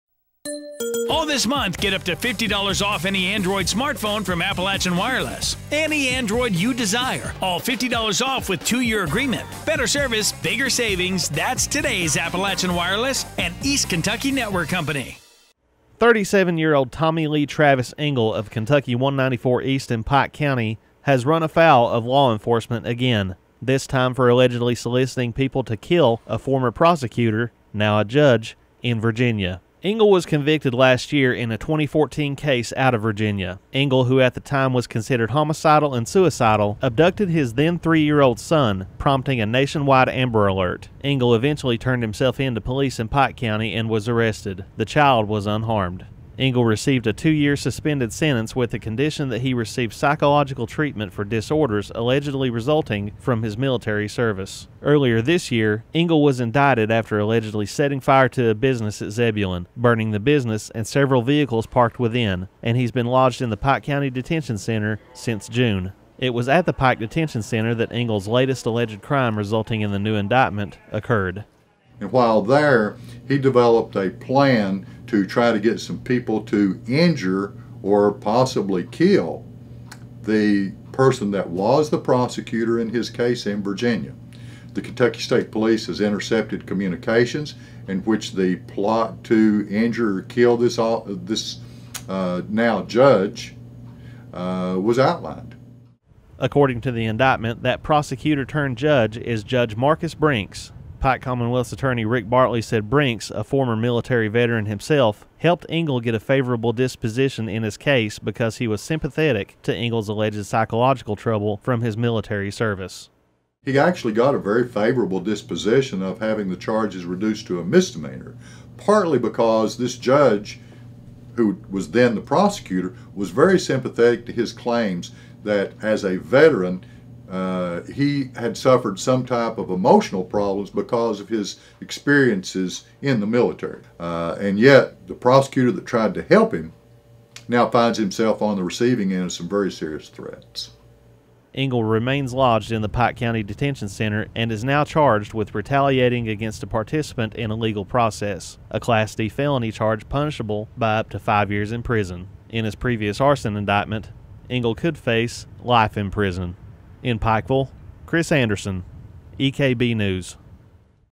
Pike man accused of conspiring to kill Virginia judge
A Pike County man already under indictment on arson charges was indicted again this week, this time for allegedly conspiring to kill a judge in Virginia. EKB News Reporter Chris Anderson has the story.